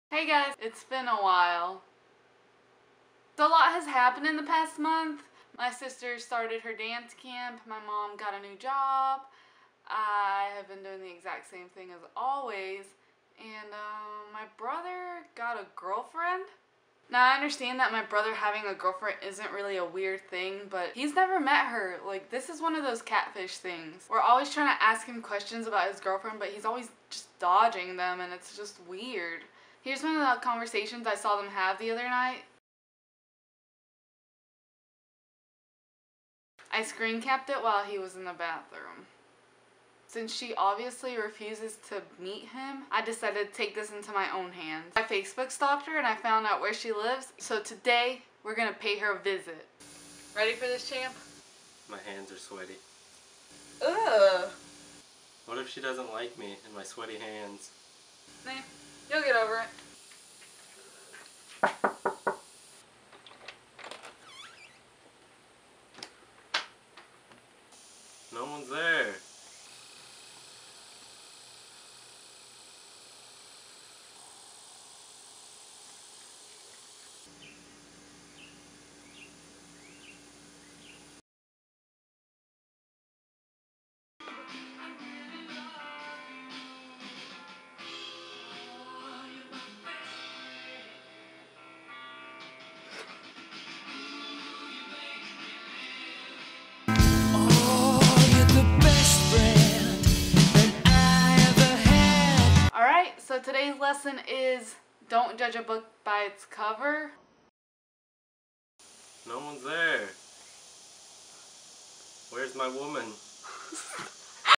Where's My Woman? DELETED MTV CATFISH EPISODE
Vlog 29: Here's a deleted episode of Catfish. Just kidding. Here's
actually a catfish inspired video. I haven't actually watched the show
though...